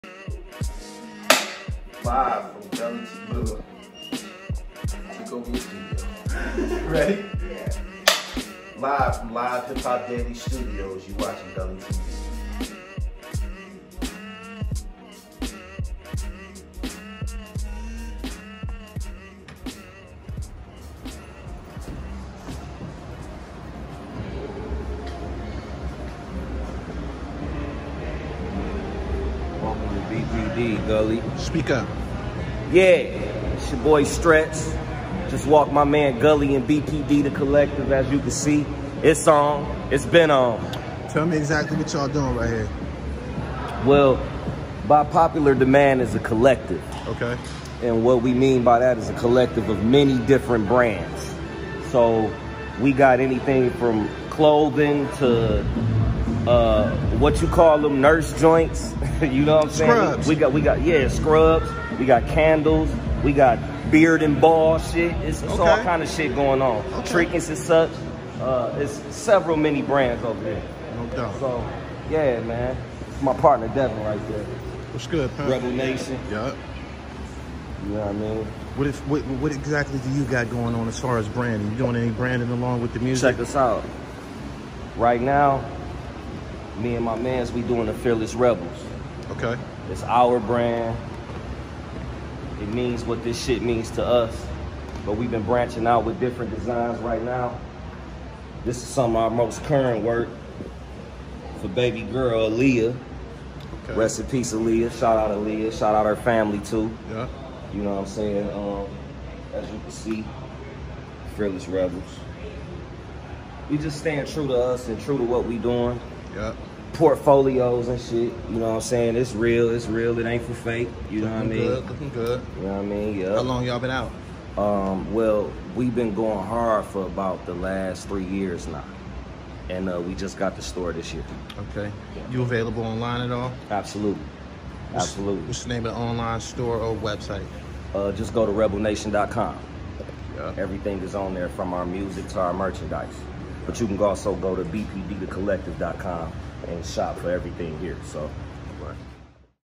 Stretch Money gives Gully Tv a tour of BPD Collective & introduces his brand “Fearless Rebels”
BUY MY NEW BOOK
"SOMETHING'S I BEEN THRU"

FOLLOW US ON ALL SOCIAL MEDIA PLATFORMS AND OUR ONLINE MAGZINE